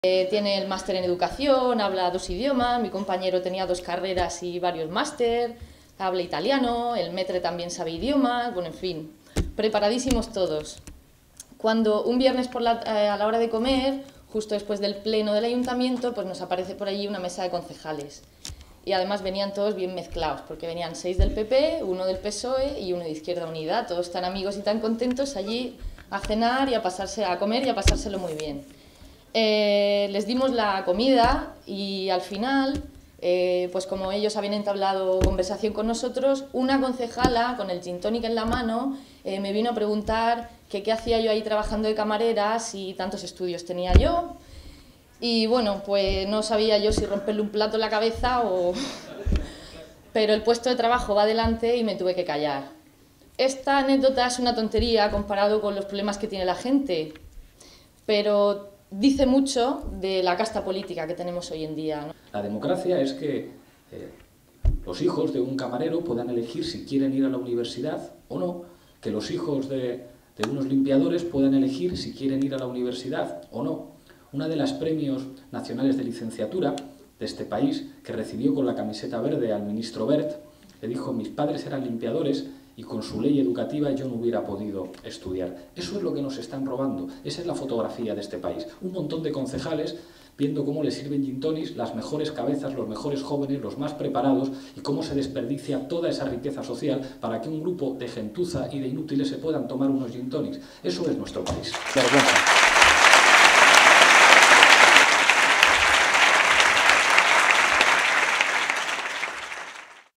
Lola Sánchez y Pablo Iglesias: ''La casta y la gente'' -
Lola Sánchez (candidata a las primarias de Podemos por el Círculo de Cartagena) y Pablo Iglesias (candidato a las primarias de Podemos por el Círculo de la UCM) intervienen en el acto de Podemos en Murcia.